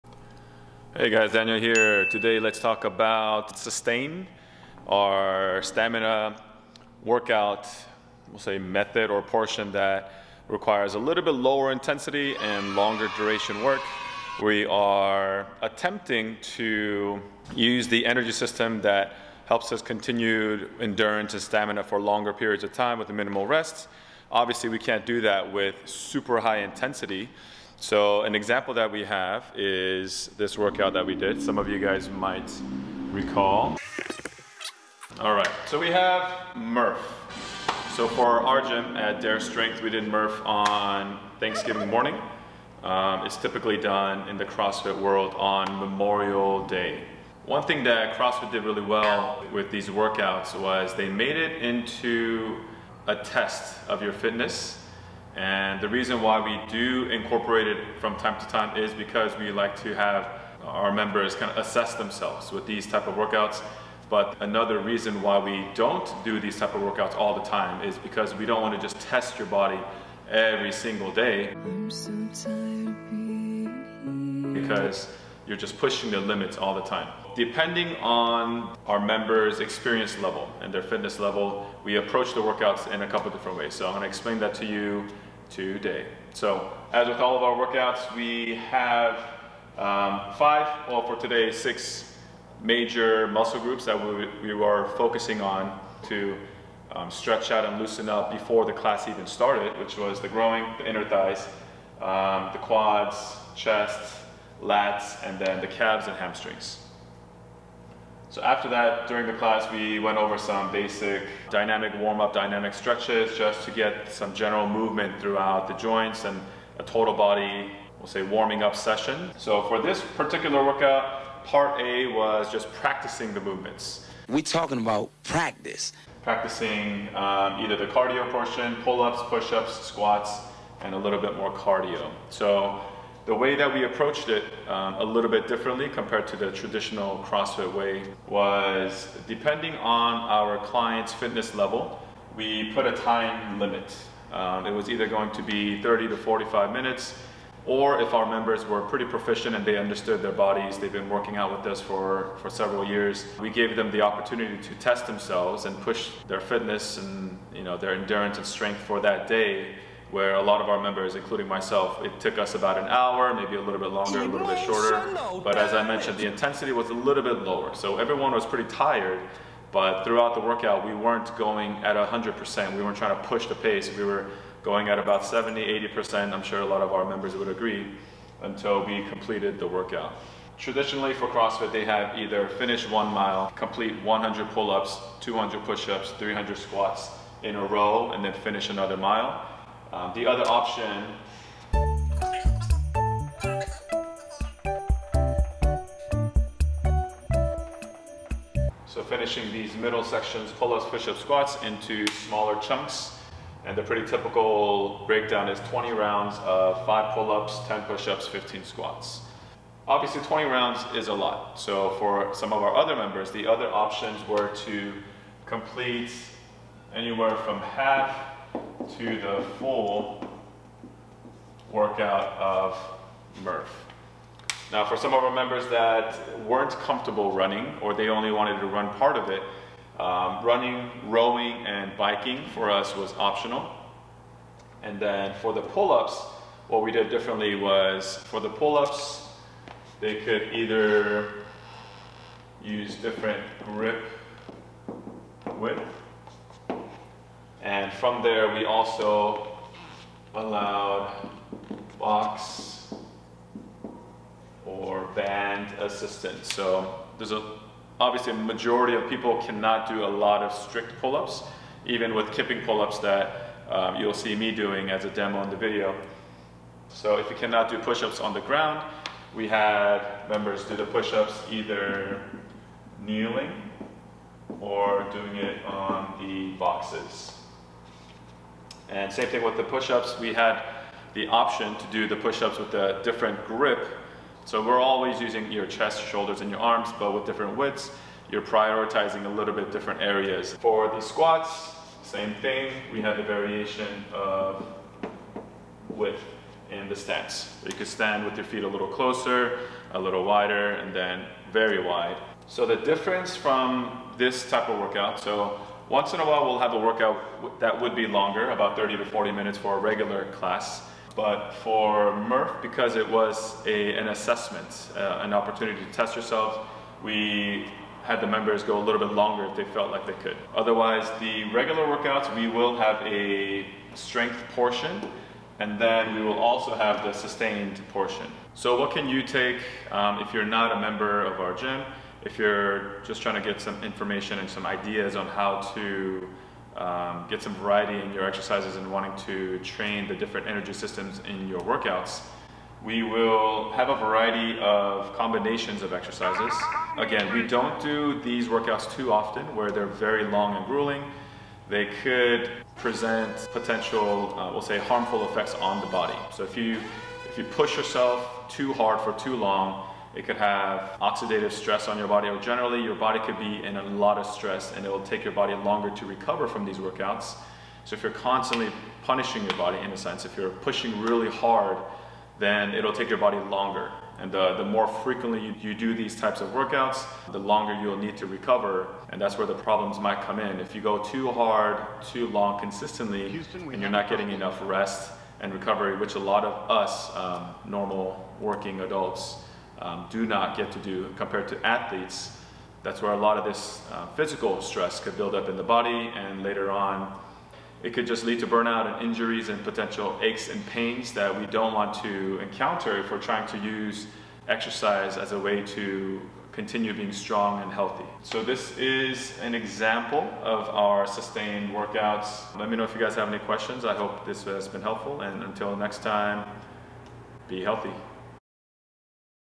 Sustain (Murph) Workout Sample
Here's look into a workout I programmed for our group fitness class at Dare Strength.

There's usually a Strength component in our workouts but considering the length of this workout... we just had a Stamina component.
One of our Stamina Variations is SUSTAIN.

We did the CrossFit Workout MURPH, in a way that is adjustable to average people that have varying level of fitness.

I encourage roughly 70% effort throughout our Sustain focus workouts..

We include rest periods that vary between:
- Work to Rest ratio of…
- 3-8 minutes of work followed by 1-2 minutes of rest

This is one way I help instill the habit of listening to one’s body while being consistent with workouts.

Sustain is a variation of our Stamina section where we focus on moving more at a steady pace to train this specific energy system in our bodies that allow for longer periods of work / stamina.